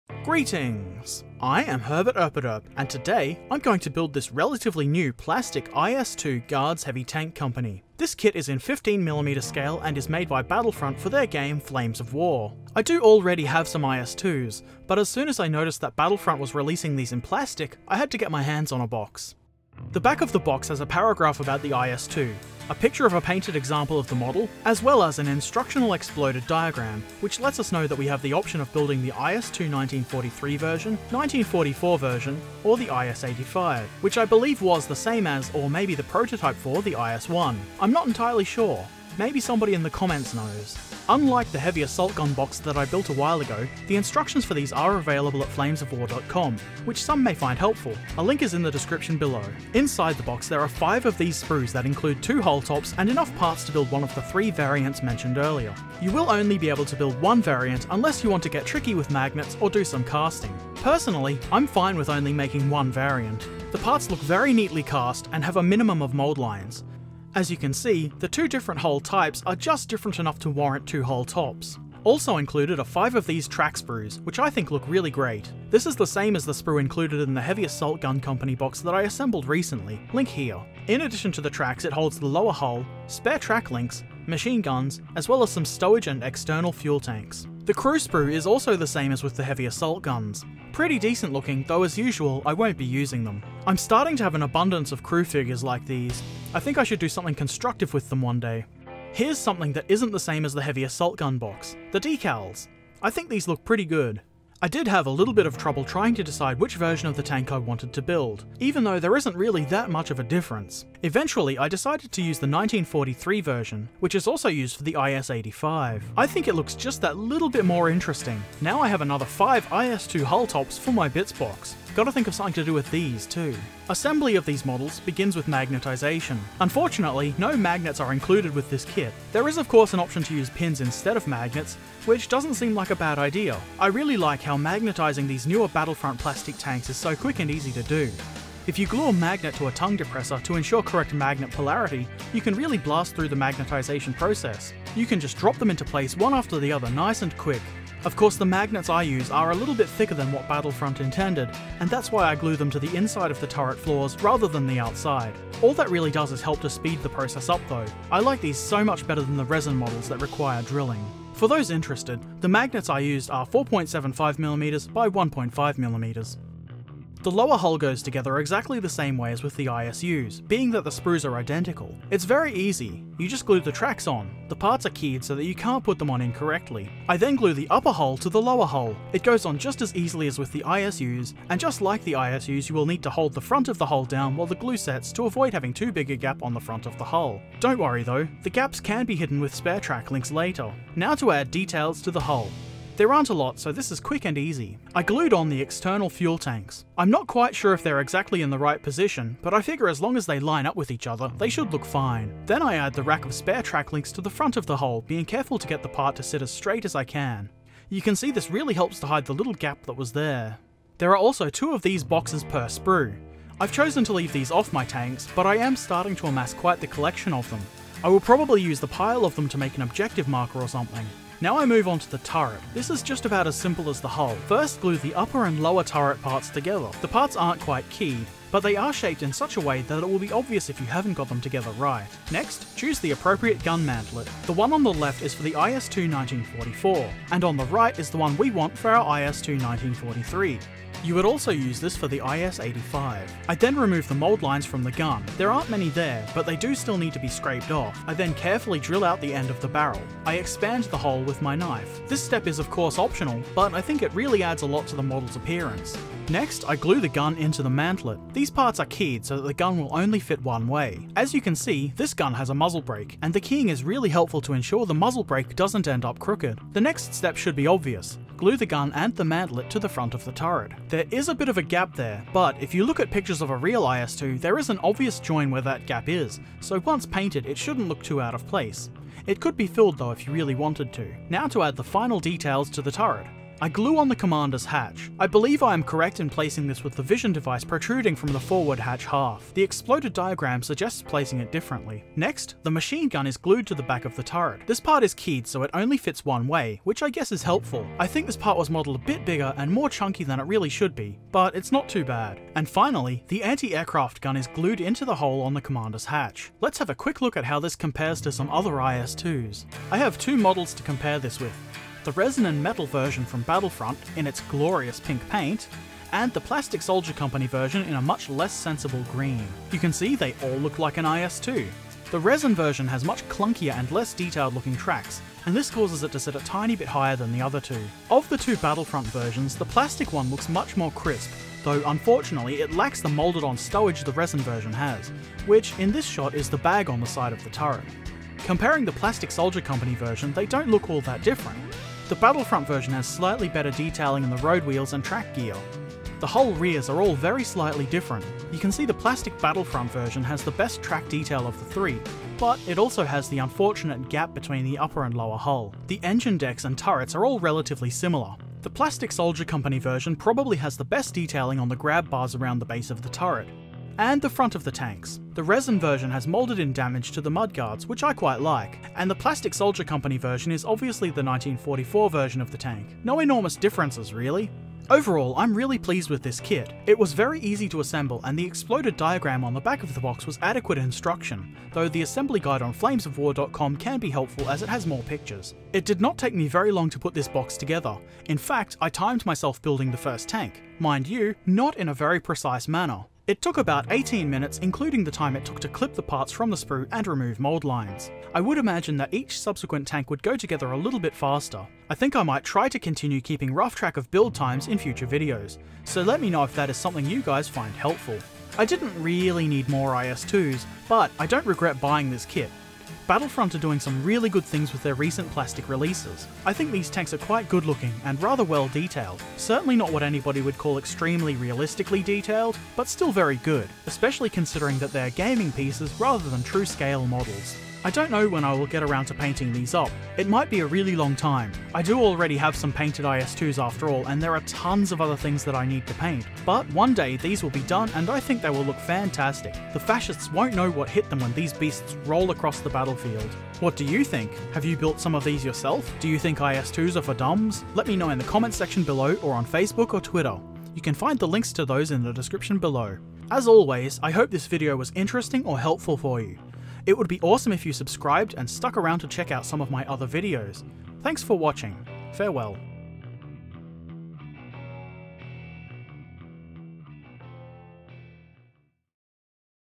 Flames of War Plastic IS 2 kit assembly review
Here are some rather nice plastic IS-2 miniatures for Flames of War. I compared them with a resin/metal version and Plastic Soldier Company's plastic IS-2.

If you want to tell me how crap at games I am (I don’t actually care.) or anything else you can leave a comment below, or find me on the following social media things.